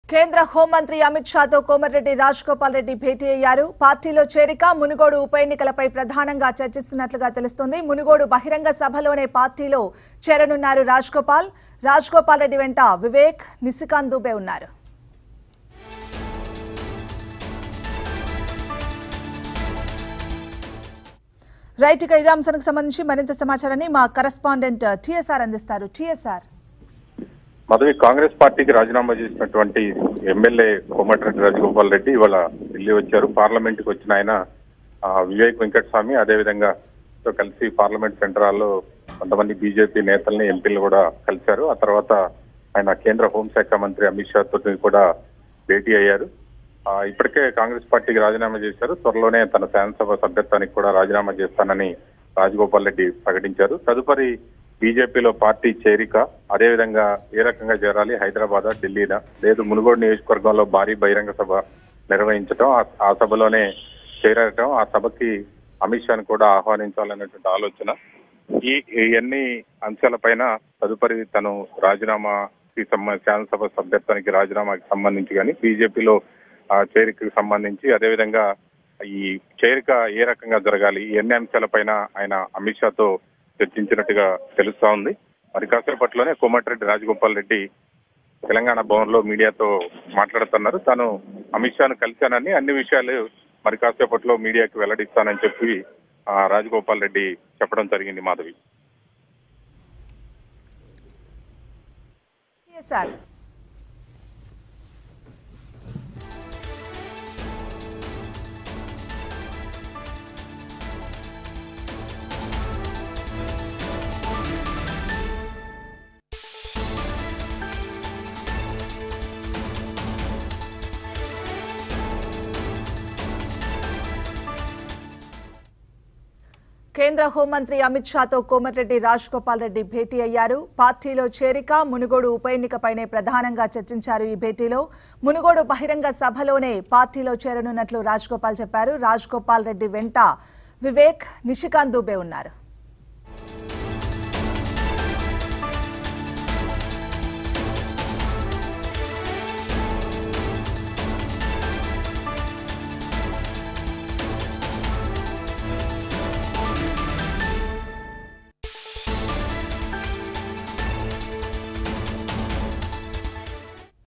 Komatireddy Rajagopal Reddy Meet With Union Home Minister Amit Shah | TV5 News Digital
బీజేపీలో చేరిక పై అమిత్ షా తో
రాజగోపాల్ రెడ్డి కీలక భేటీ..